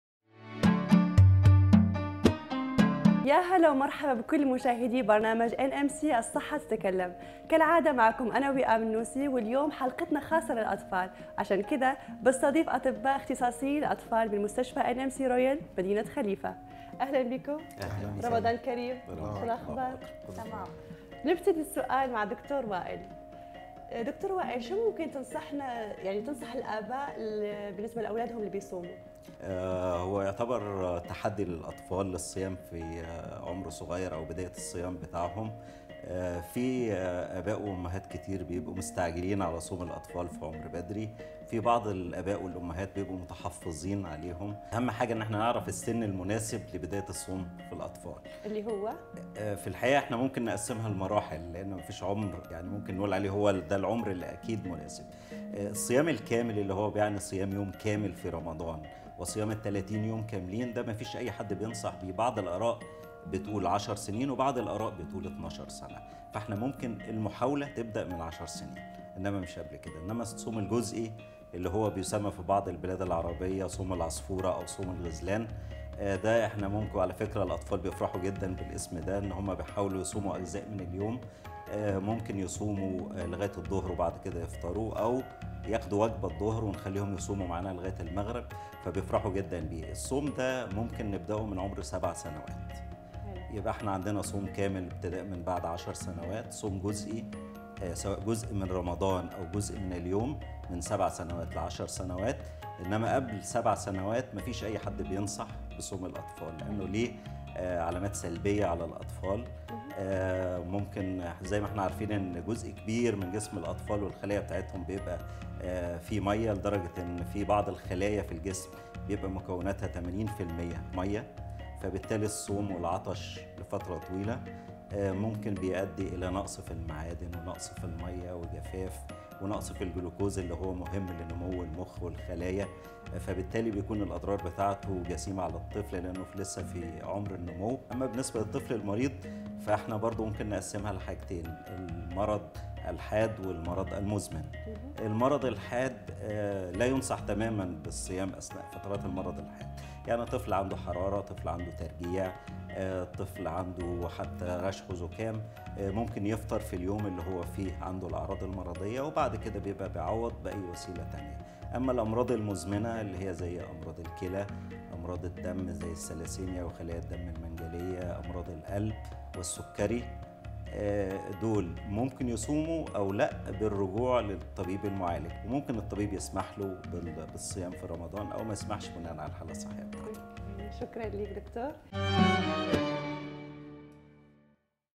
Episode 6 of NMC Health Talk - Ramadan Special
In this episode of NMC Health Talk, pediatricians from NMC Royal Hospital, Khalifa City address some very important aspects about young children fasting during the Holy month of Ramadan.

في هذه الحلقة من برنامج "إن إم سي الصحة تتكلم"، يتناول أطباء الأطفال من مستشفى إن إم سي رويال بمدينة خليفة بعض الجوانب المهمة للغاية حول صيام الأطفال الصغار خلال شهر رمضان المبارك.